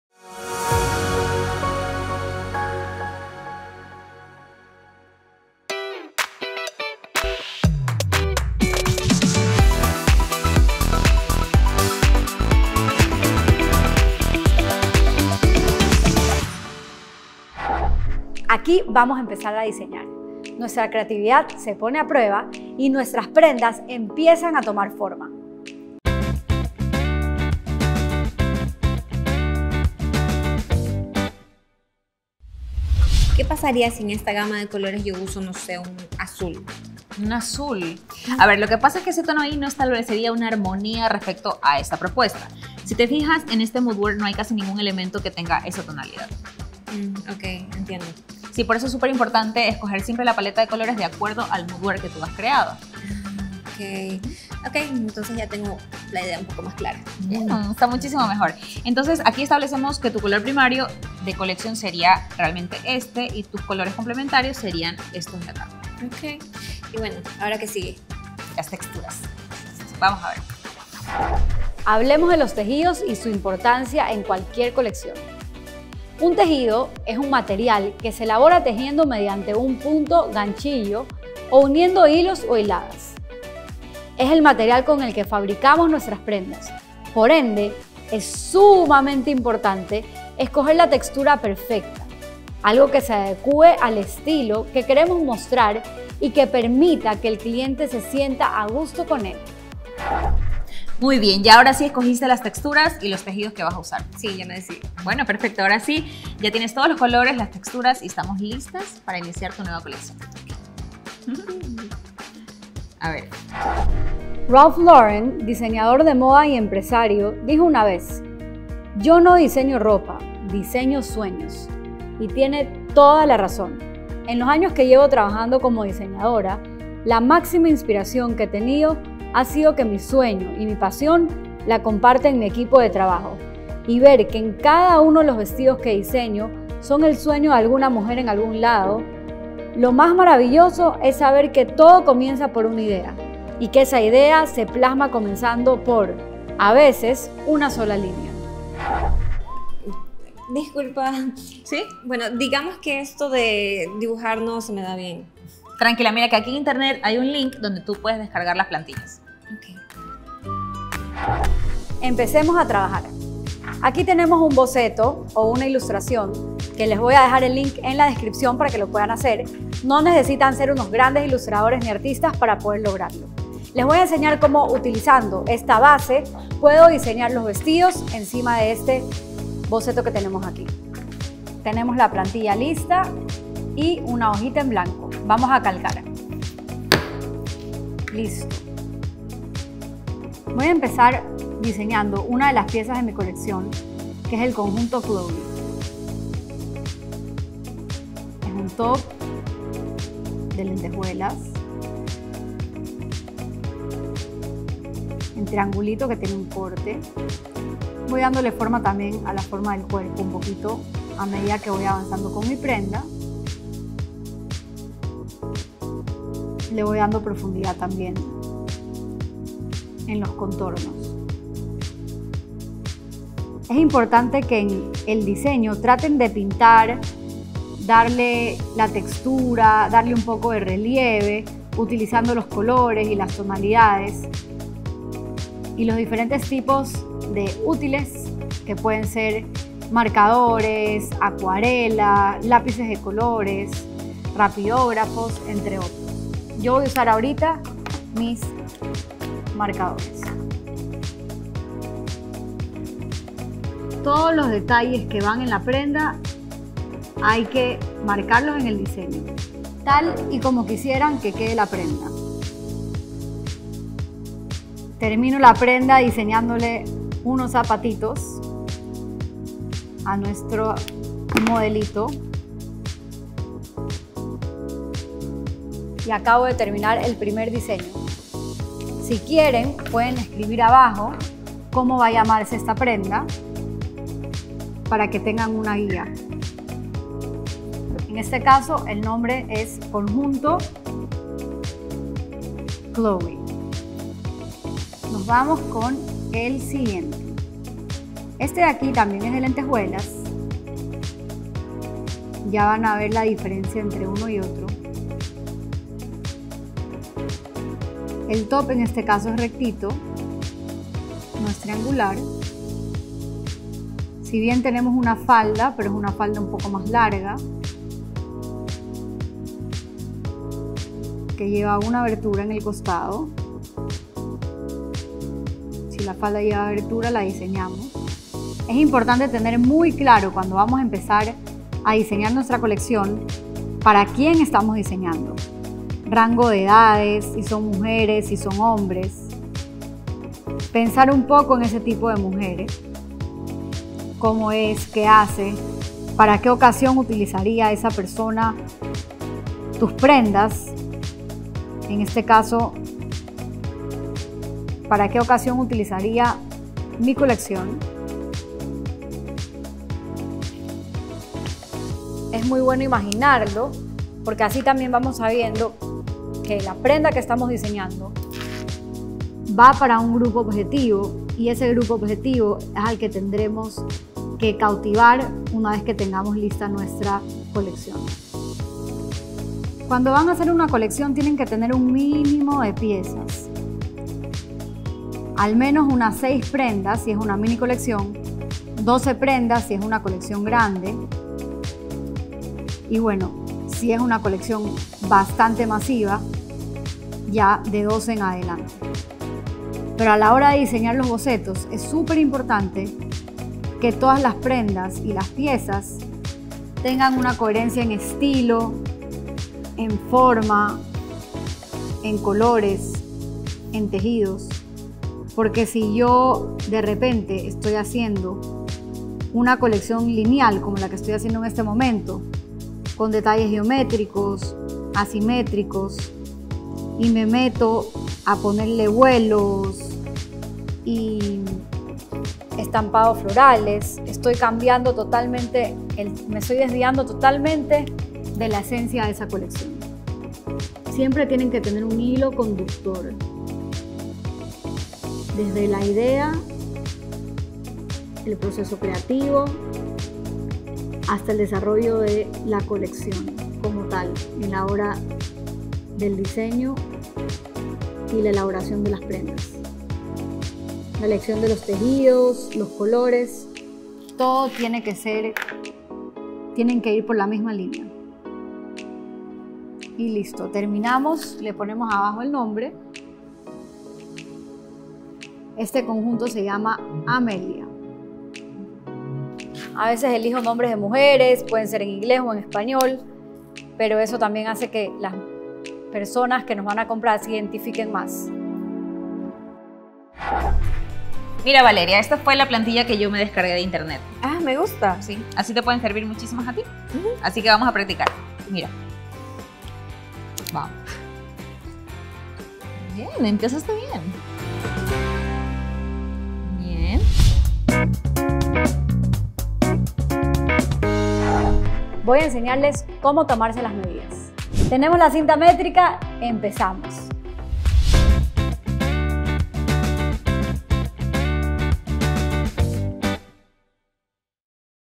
Moda ep 4 - Guayaquil Aprende Online
Curso de Moda con Isabella Salvador - Capítulo 4.

Proyecto impulsado por ZUMAR de la Alcaldía de Guayaquil.